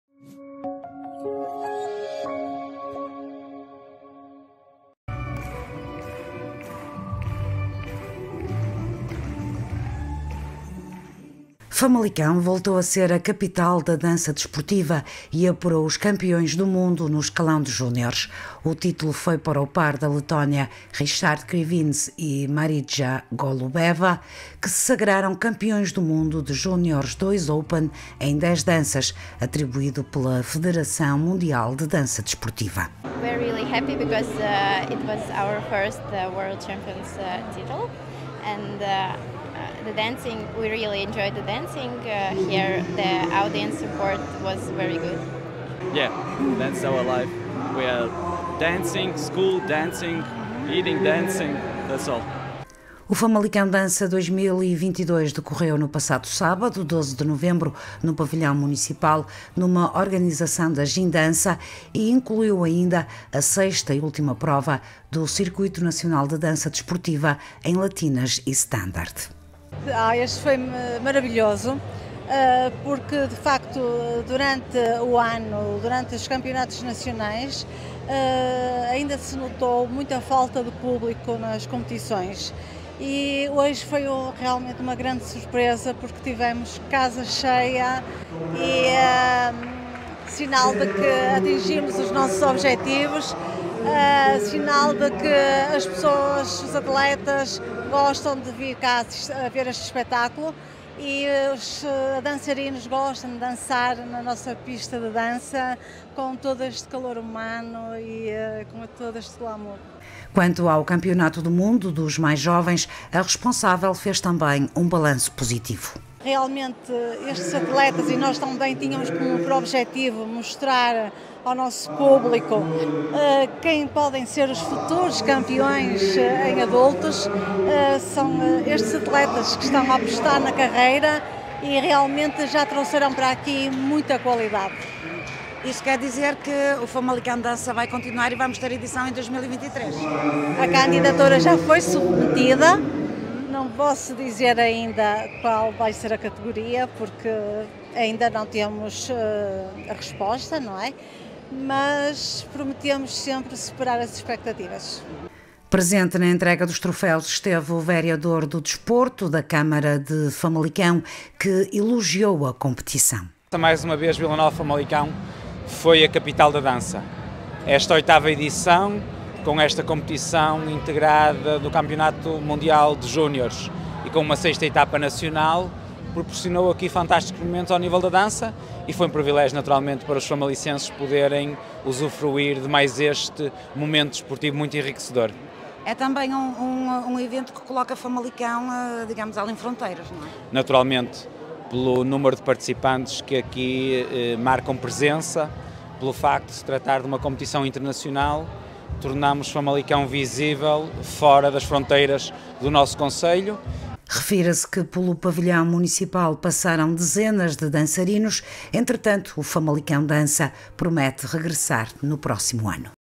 Richard Krivins e Marija Golubeva da Letónia, foram os grandes vencedores do Famalicão Dança 2022
Famalicão voltou a ser a capital da dança desportiva e apurou os campeões do mundo no escalão de juniores.
O título foi para o par da Letónia, Richard Krivins e Marija Golubeva, que se sagraram campeões do mundo de Juniores 2 Open em 10 Danças, atribuído pela Federação Mundial de Dança Desportiva.